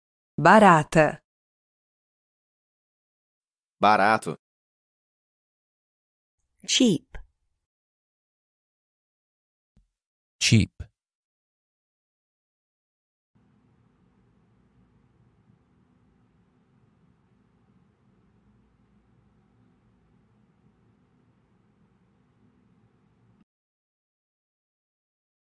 How to say "Cheap" in Portuguese
Male Voice:
Barato

Female Voice:
Barata

Male Voice Translation:
Cheap

Female Voice Translation:
Cheap